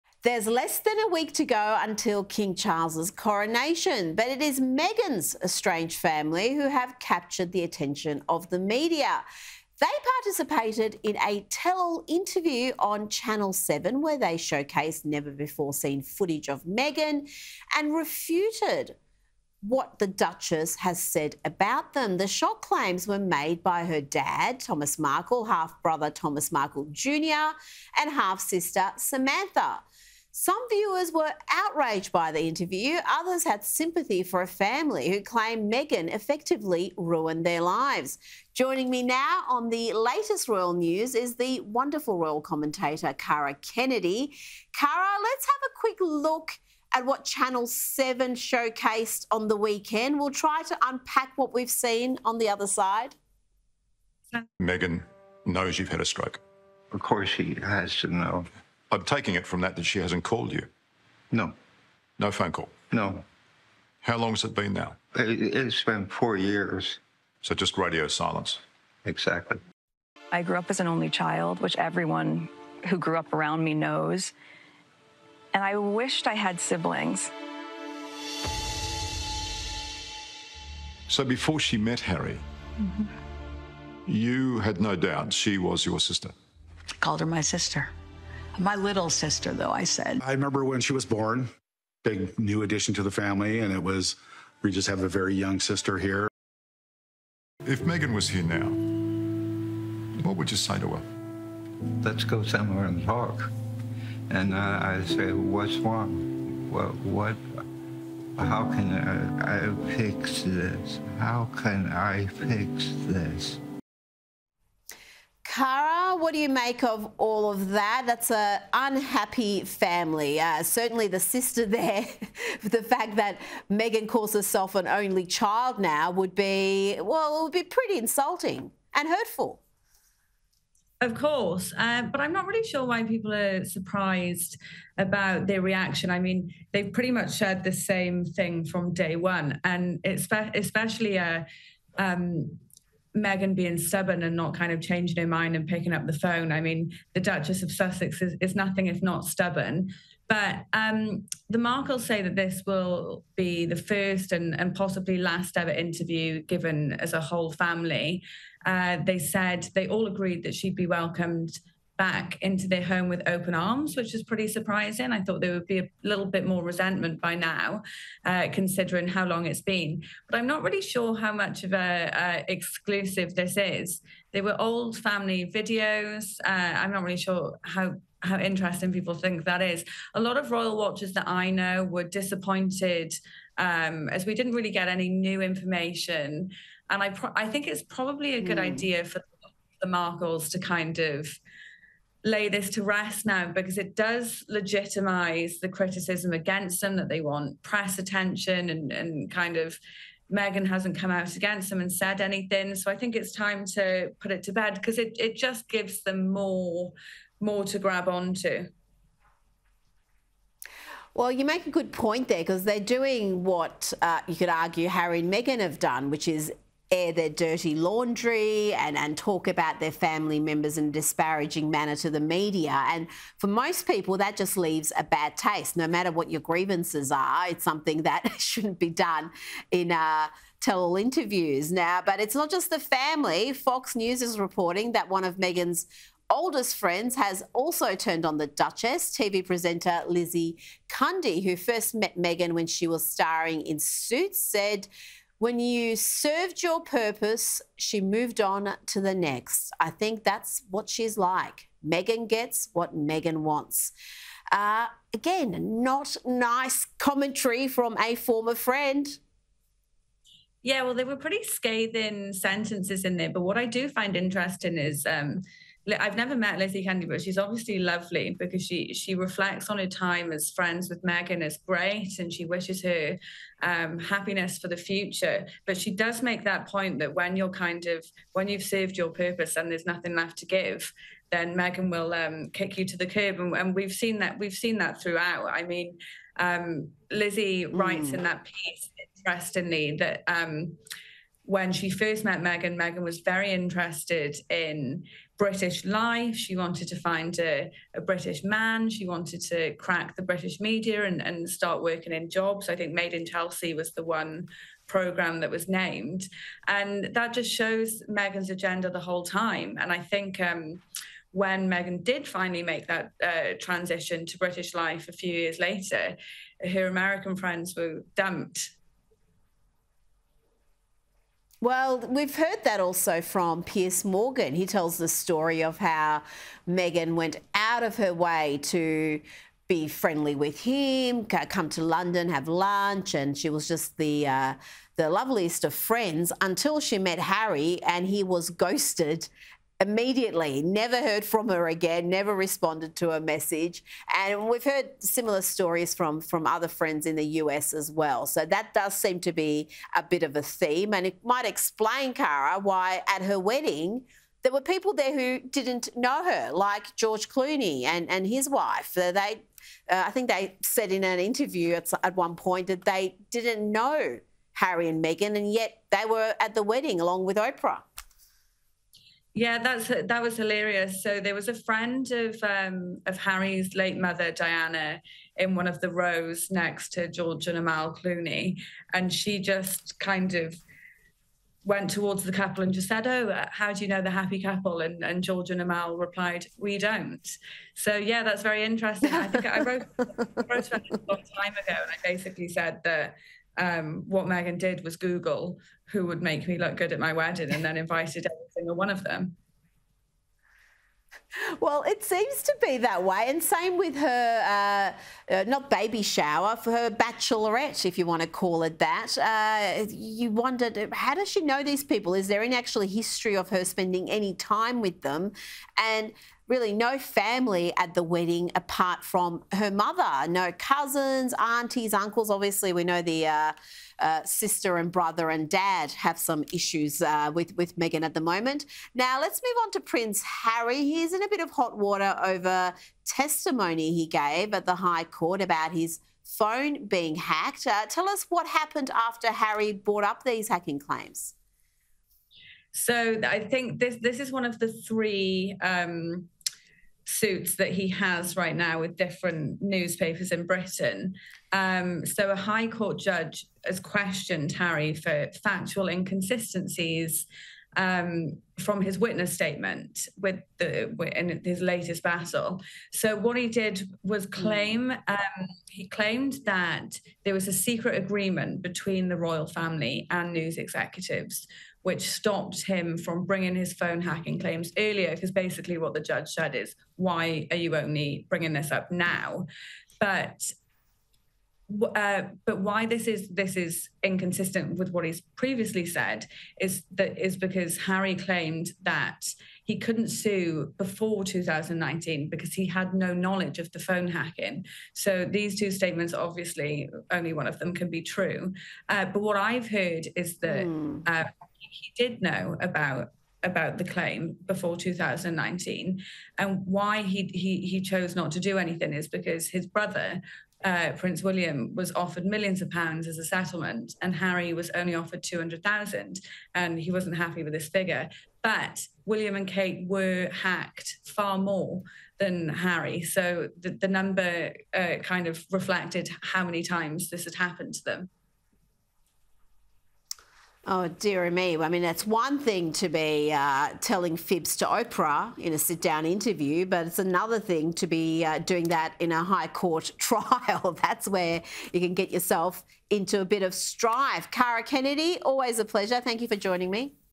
Meghan Markle ‘dumped’ American friends after move to UK
Royal commentator Kara Kennedy says it seems clear Meghan Markle’s friends from America were “dumped” after she moved to the UK to marry Prince Harry.